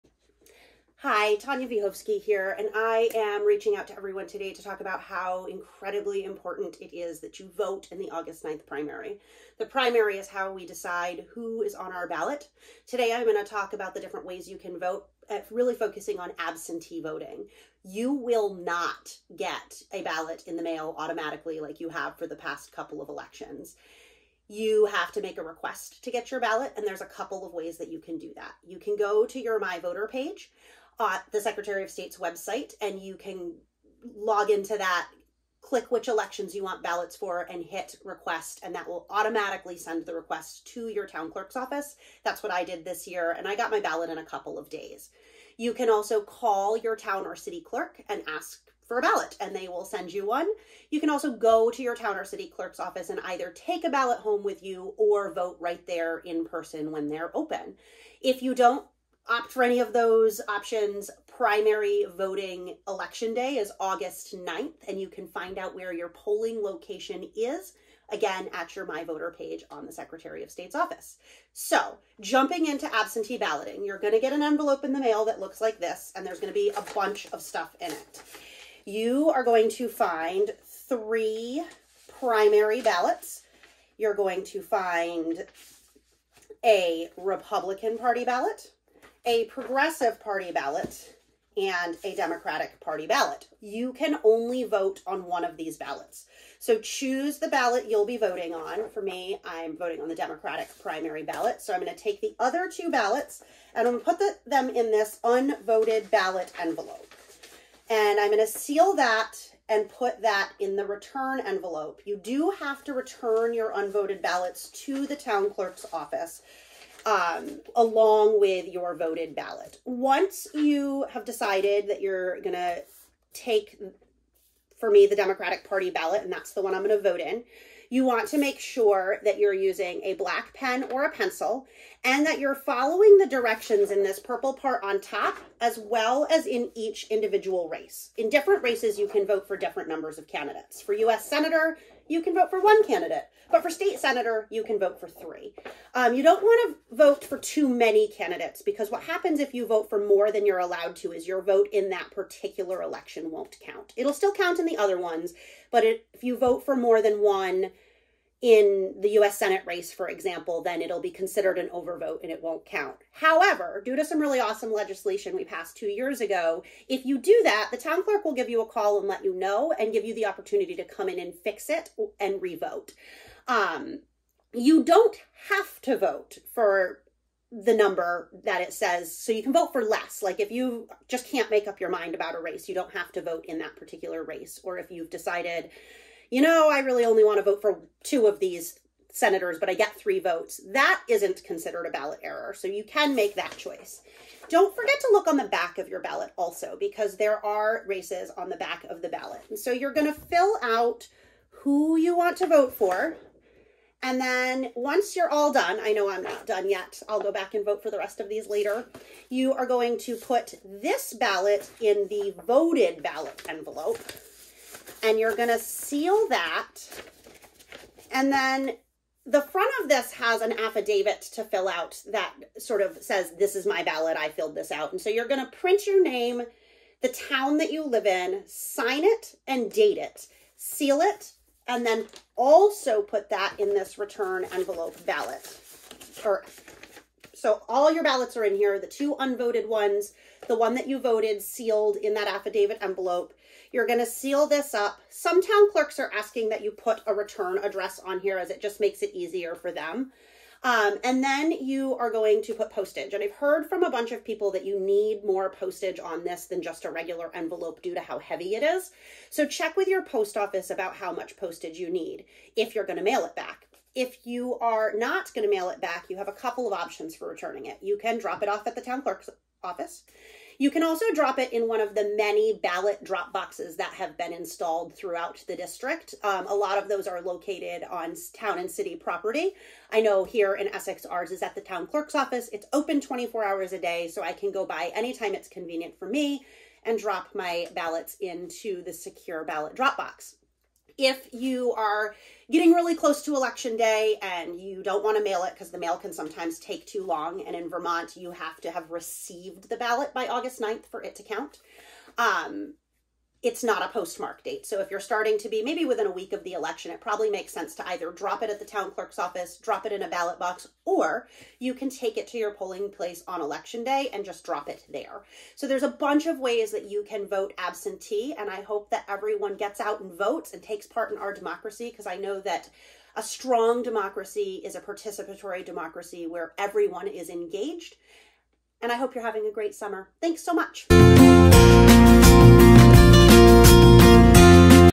Voting in the Vermont 2022 Primary
Step-by-step instructions on how to request and vote by absentee ballot in the Vermont 2022 primary election.